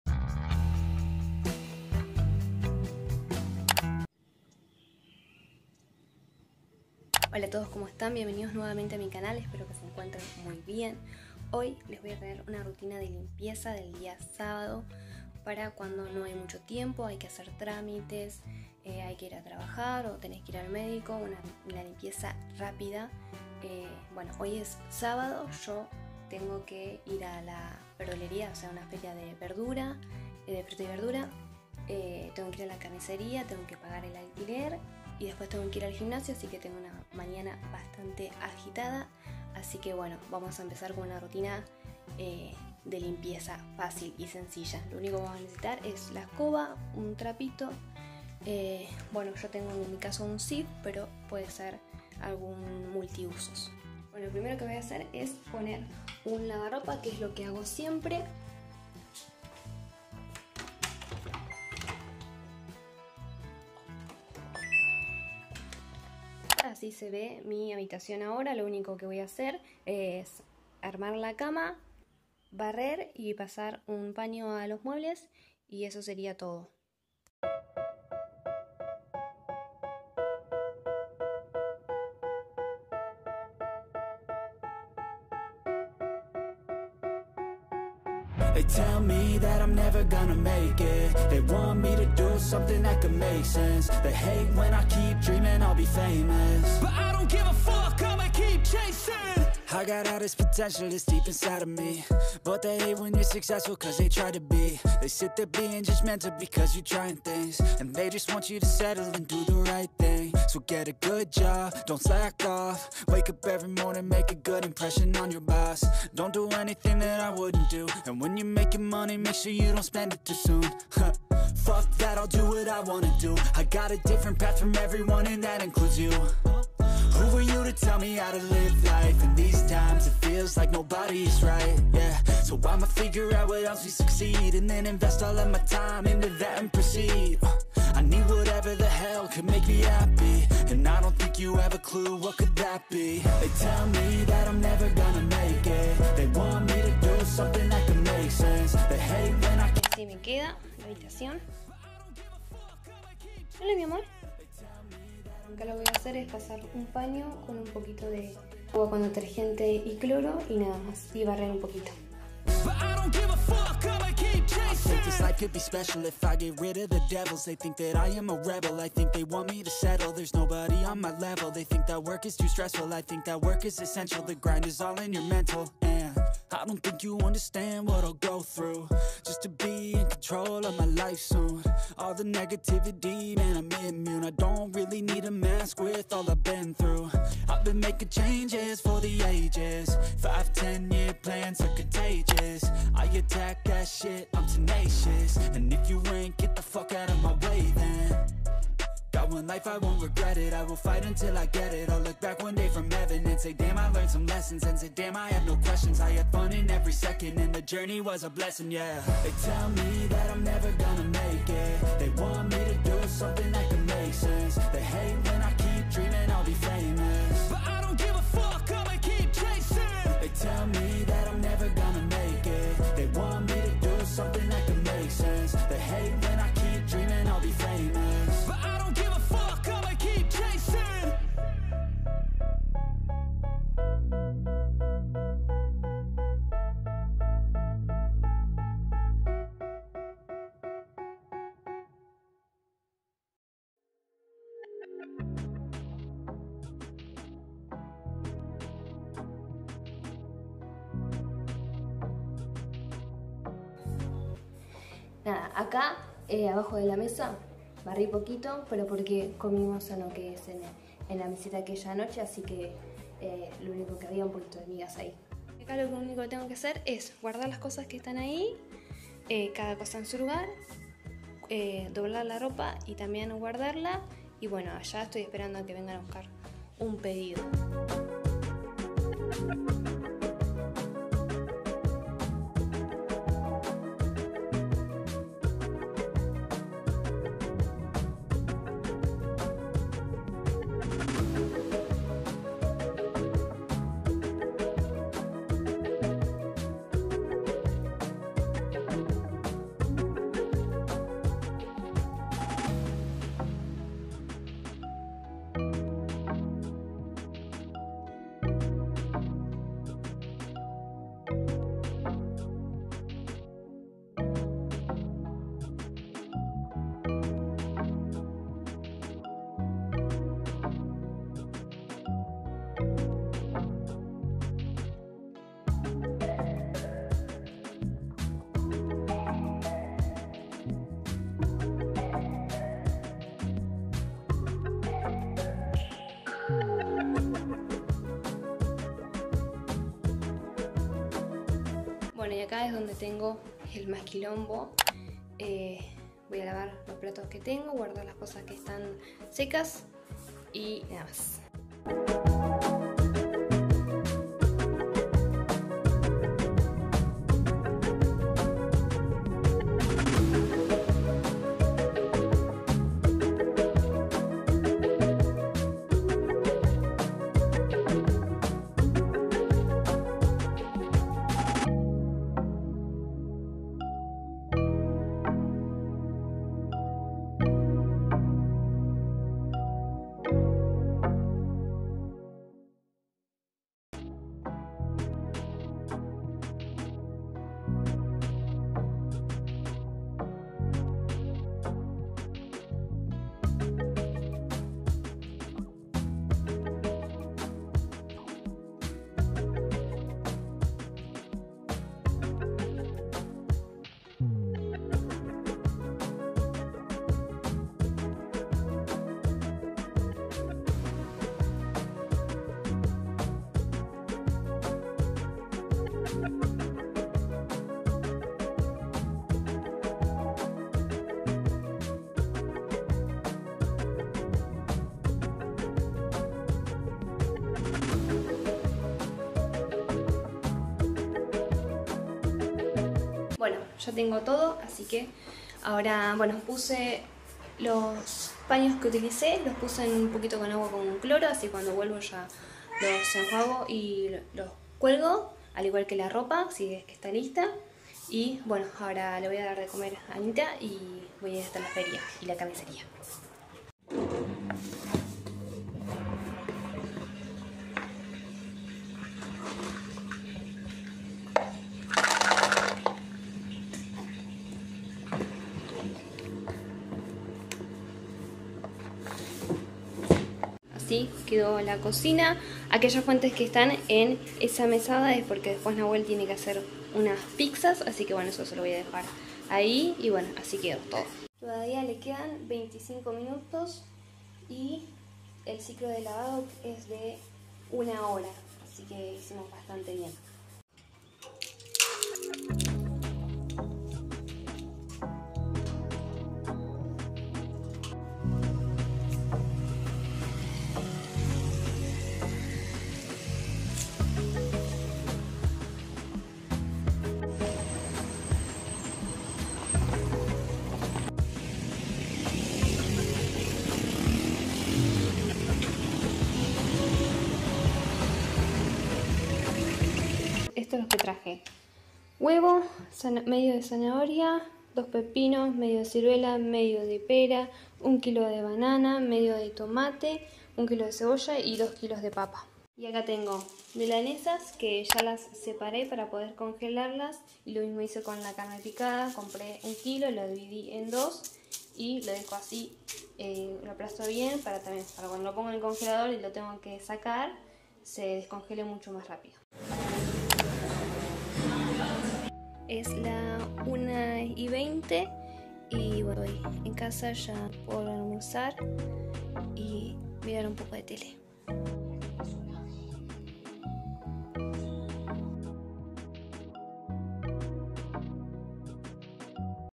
Rutina de Limpieza 30min
Buen Lunes para todos, espero que se encuentren muy bien. Hoy les traigo una rutina de limpieza para cuando hay poco tiempo y parte de mí día sábado espero que les guste.
saludos